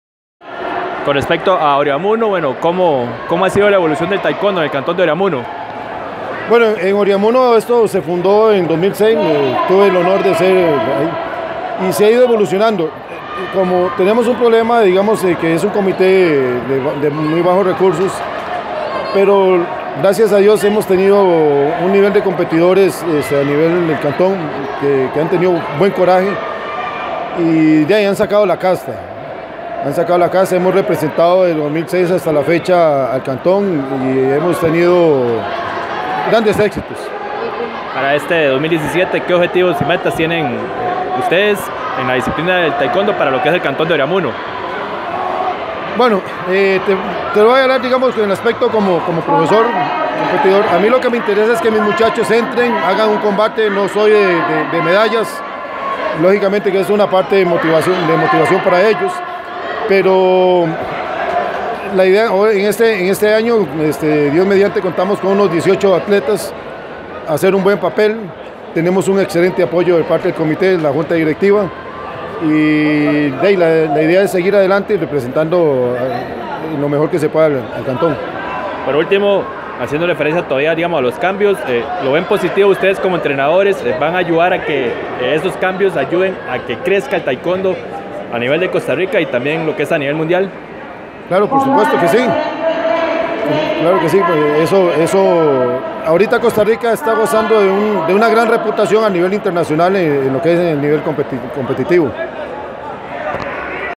Álvaro Barahona Soto, técnico del equipo de taekwondo de CCDR Oreamuno
El estratega habla sobre el crecimiento de esa disciplina en dicho cantón y cuáles son los objetivos para este año